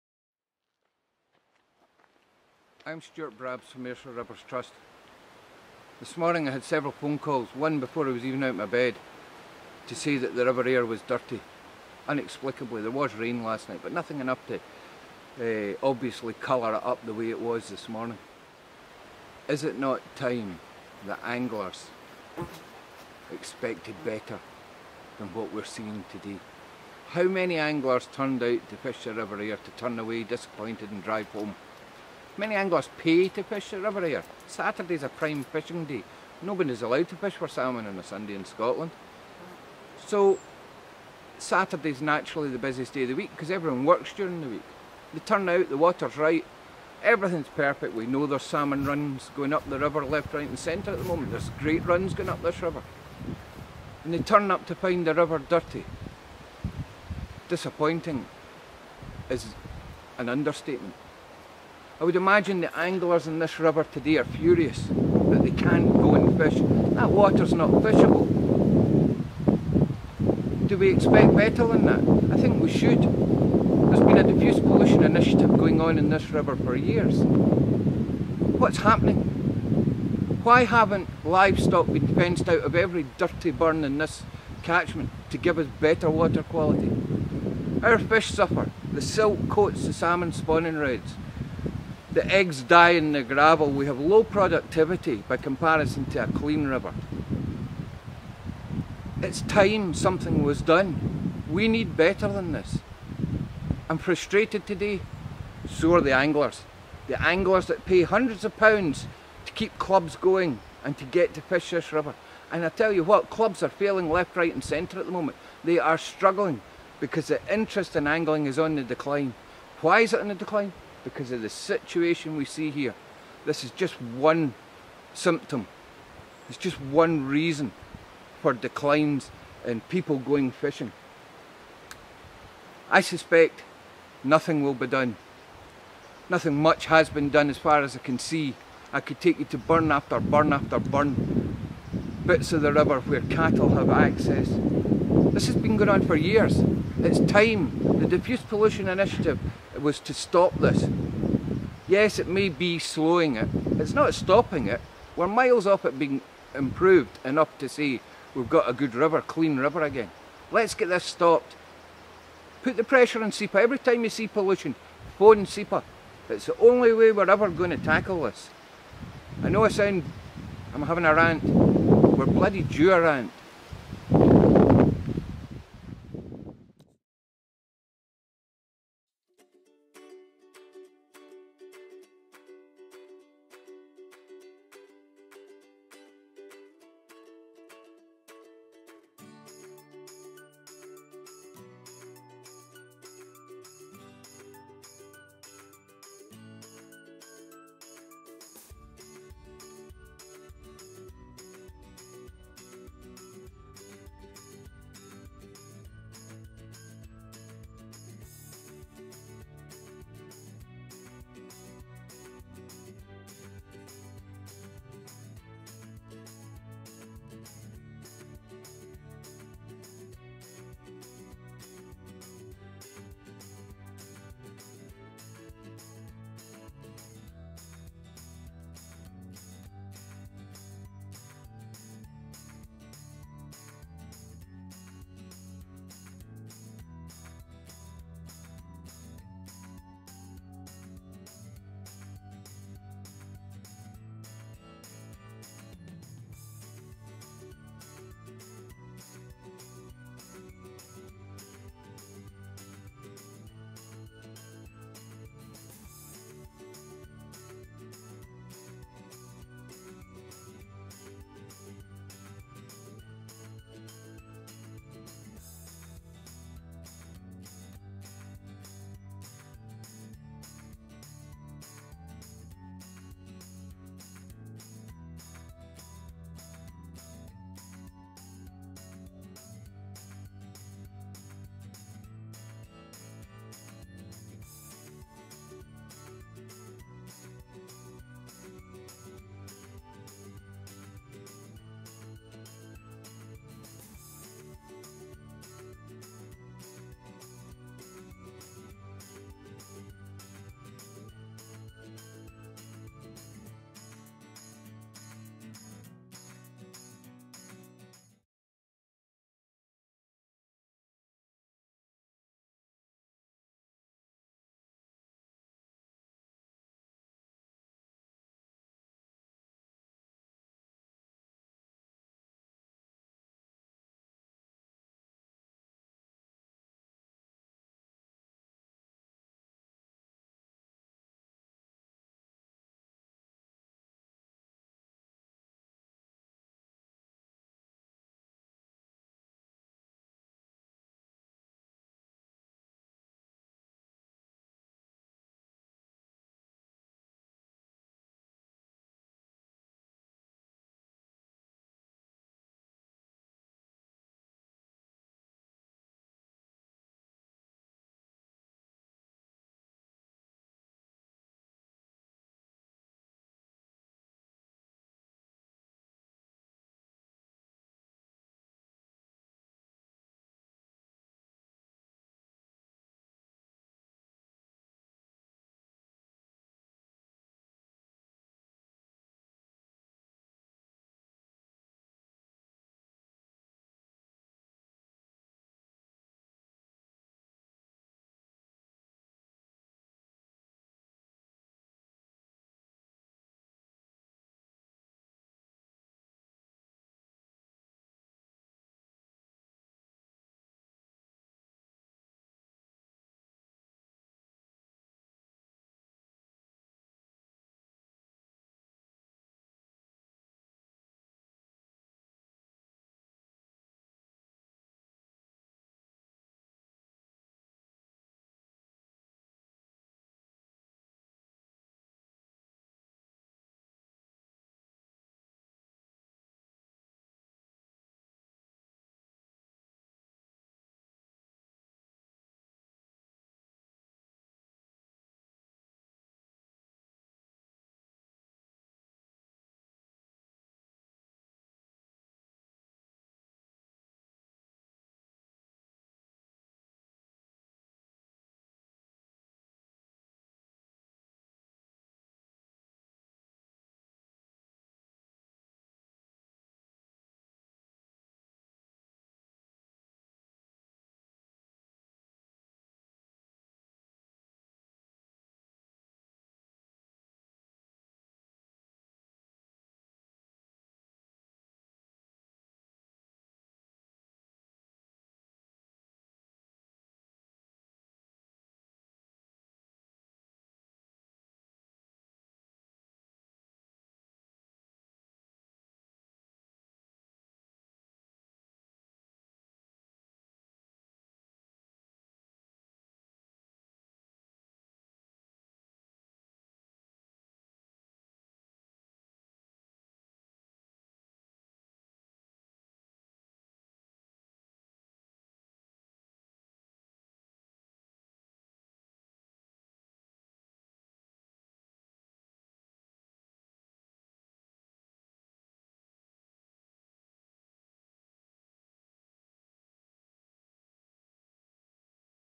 Bogend Burn Pollution
This video is about Pollution on the Bogend Burn and River Ayr